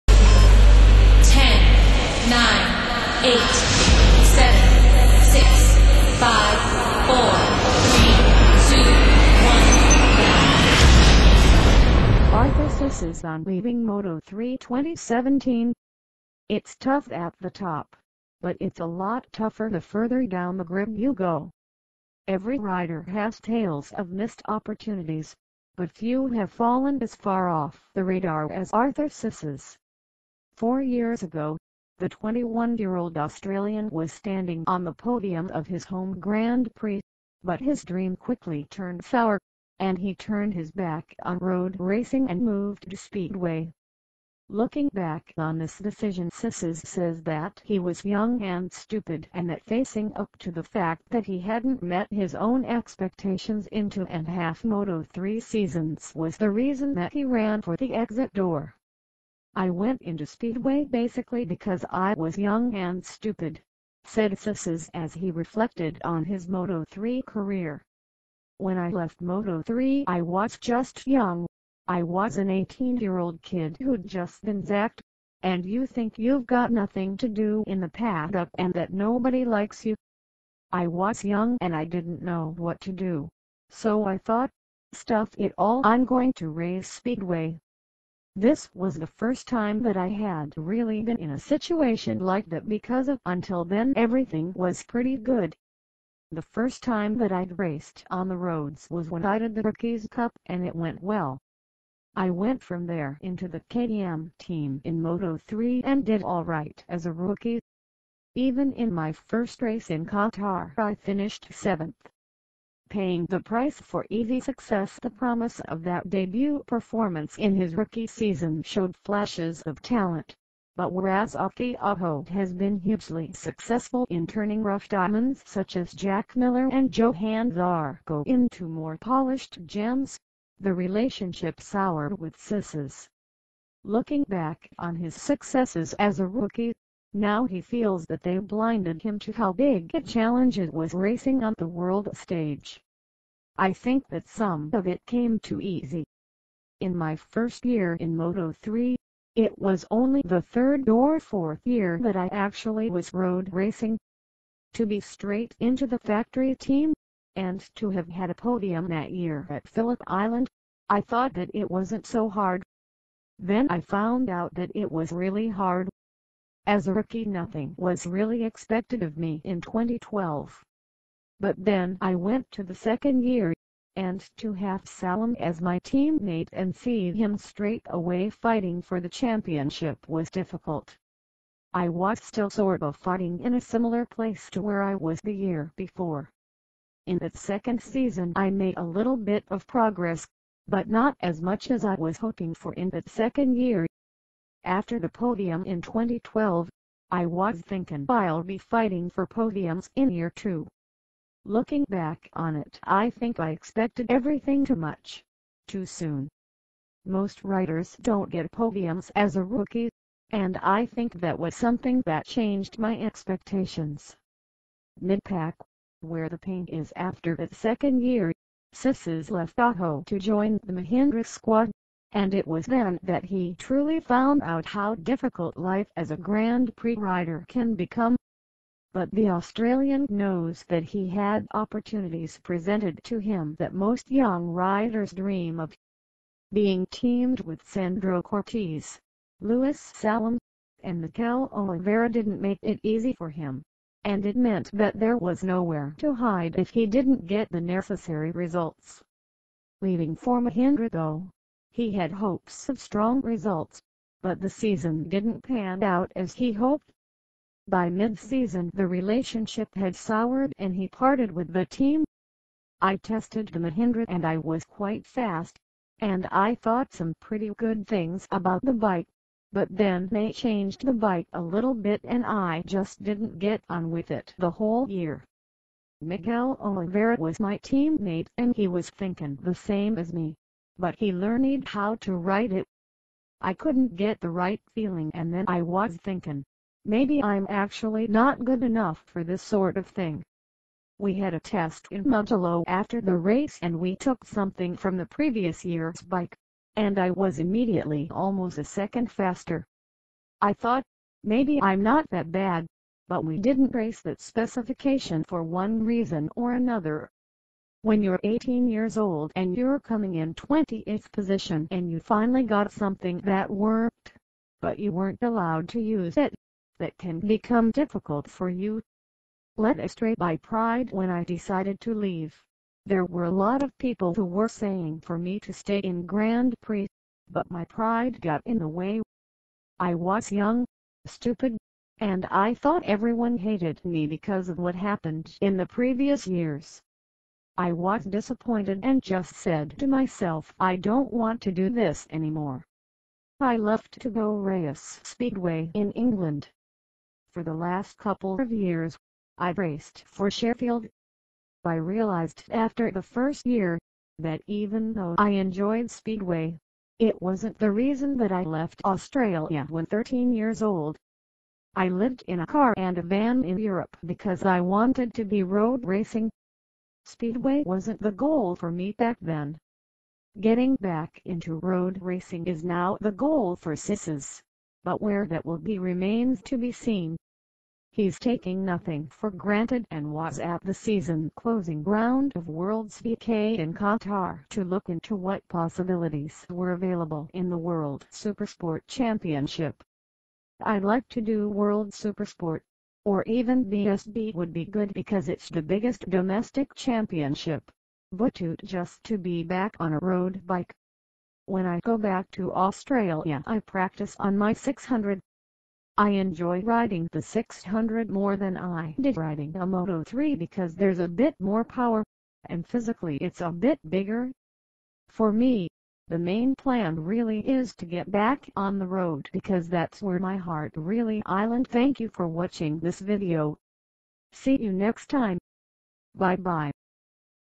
2017 Moto Gp Arthur Sissis on Leaving Moto3. It’s tough at the top, but...
[] 2017, MOTO GP, MAKI AJO, ARTHUR SISSIS, AUSTRALIA, KTM, MOTO3, RACER, RACING, SPEEDWAY